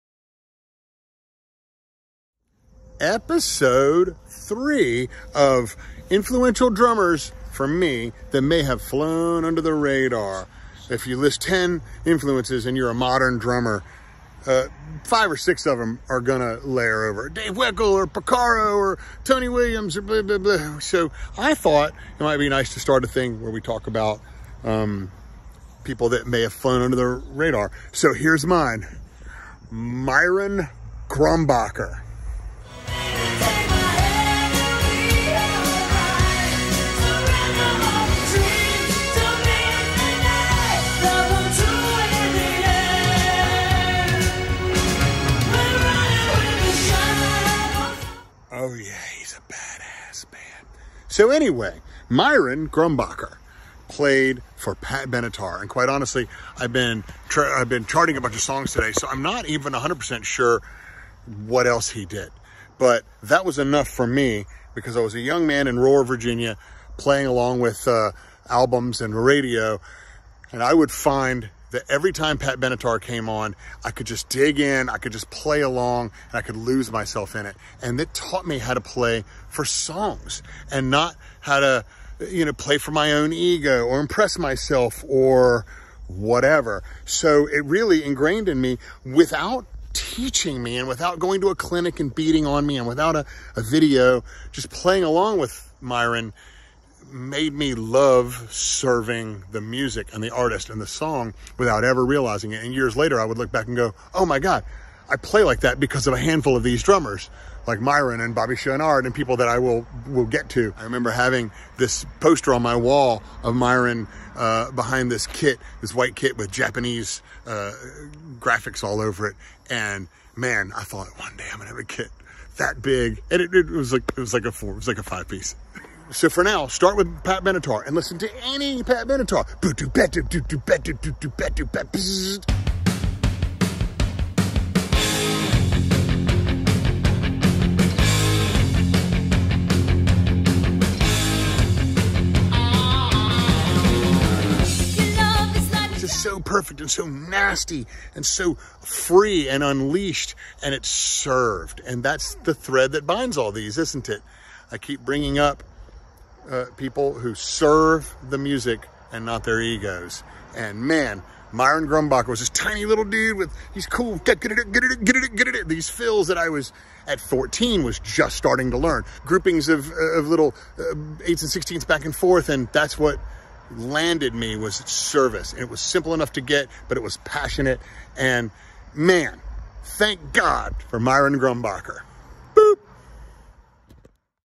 Kevin Murphy's "Under The Radar" - Myron Grombacher!
Part THREE of “my influences who might have flown under your radar!” series. I LOVE the “top 10 influences” IG challenge but unnoticed that most of my friends and heroes all pretty much agree on the big names...
I have the standard list as many, for sure, but in an attempt to pass along some quality info to younger players I’m going to post some heavyweights who may have flown under your radar... check ‘em out!